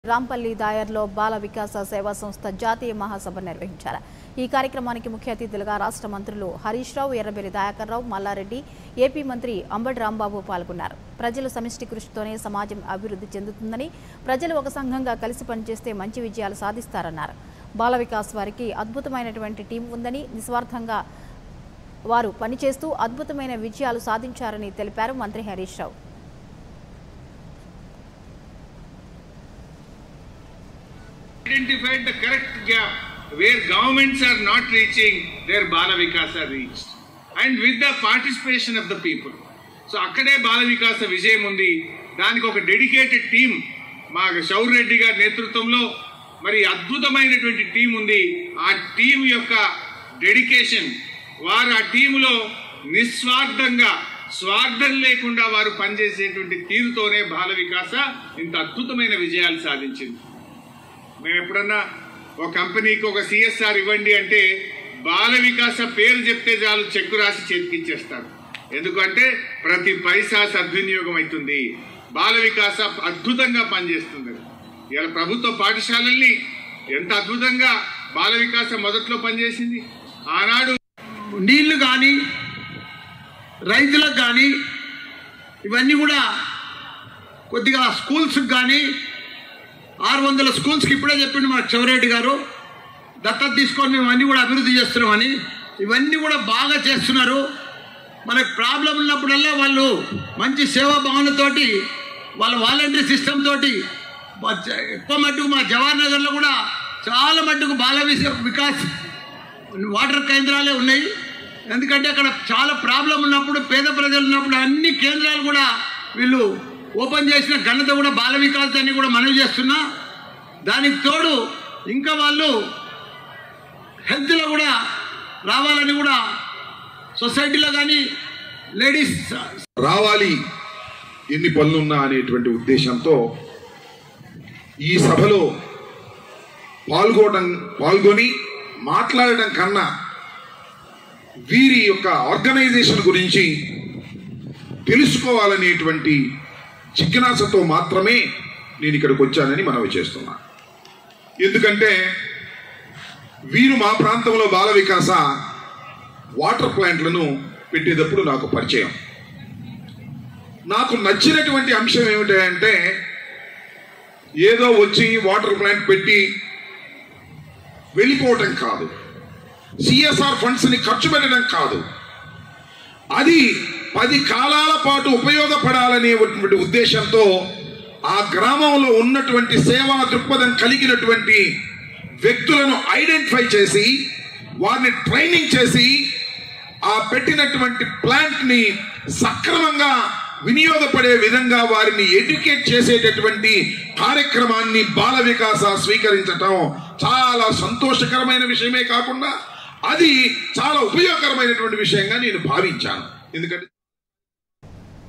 మేడ్చెల్ జిల్లా లో బాలవికాస మహాసభ..పాల్గొన్న ఏపి మంత్రి అంబటి రాంబాబు | 99TV
99TV Telugu LIVE | AP News | Telangana News | 99TV Telugu Live TV Channel